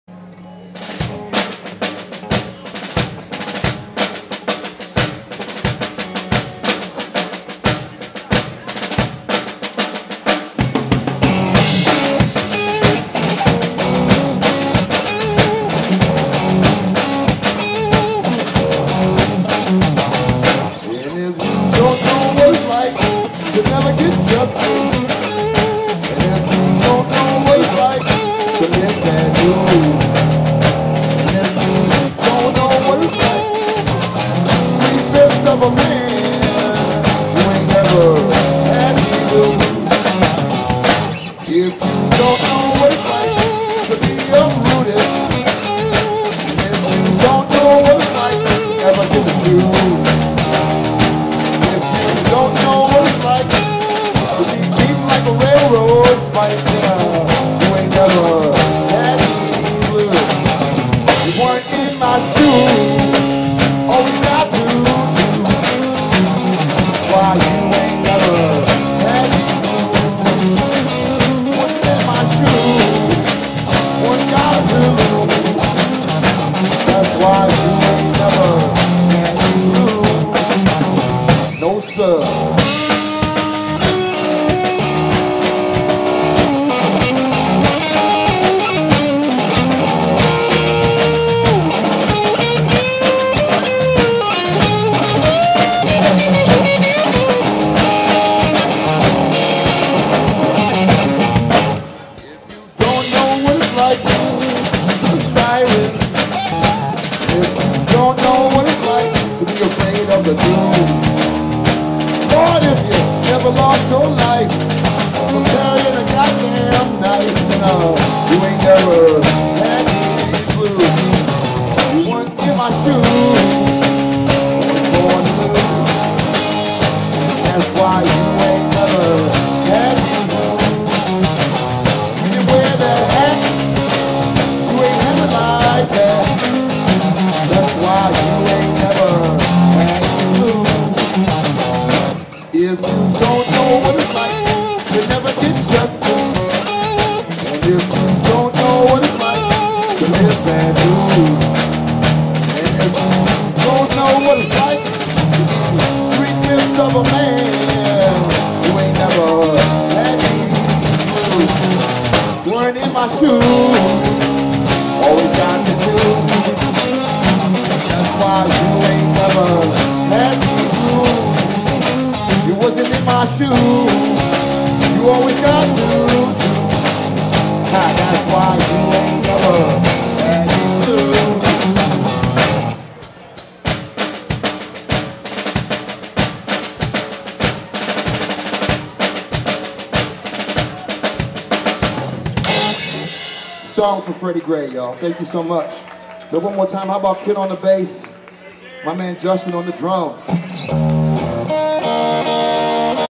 005 Bushmaster at Lexington Market 2019-01-26
Bushmaster featuring Gary Brown performed LIVE at Baltimore's historic Lexington Market on Saturday 1/26/19 from Noon till 2pm.  Such a cool vibe in the room, honest and enthusiastic response from those who came out to support live music, we appreciate you giving us your time - glad you dig our tunes!

Here is the show for you to enjoy again, share with those who may have missed it, and dance to as many times as you like  :-)

Ken Fiester - bass.
Justin PInkney - drums.

...more about the band at BushmasterBlues.com